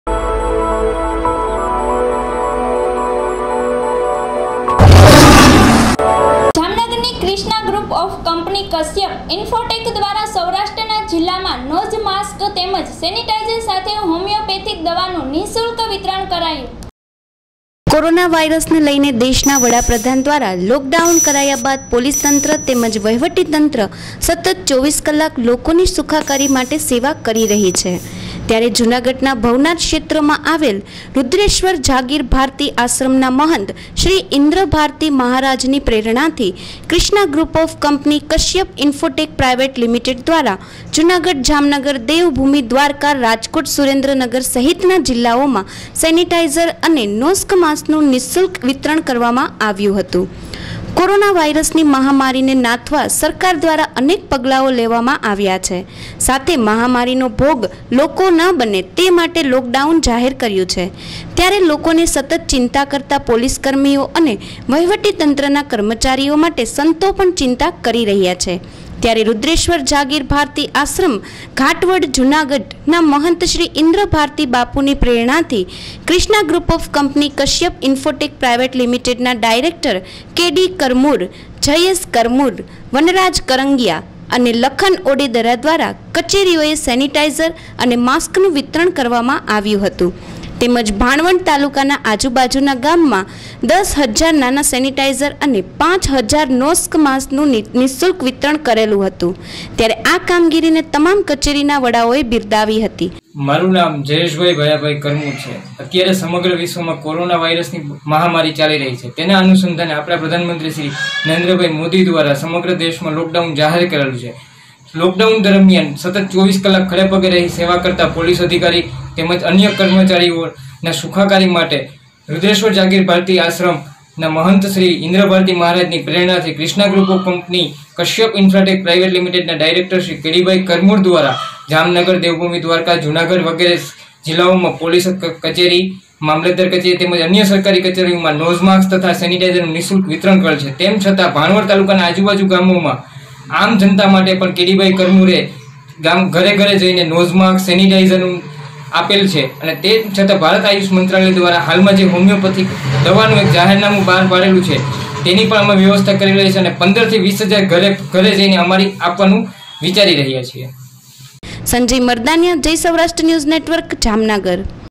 Jamnagar:જામનગર : માસ્ક તેમજ સેનેટાયજર,હોમીઓપેથીક દવાઓનું નિઃશુલ્ક વિતરણ.
જામનગર : માસ્ક તેમજ સેનેટાયજર,હોમીઓપેથીક દવાઓનું નિઃશુલ્ક વિતરણ.ક્રિશ્ના ગ્રુપ ઓફ કંપની દ્વારા ઉમદા સેવાપ્રવૃત્તિ.

રાજુભાઇ વેગડા
તંત્રી
કૌશિકભાઈ શીલુ
સહતંત્રી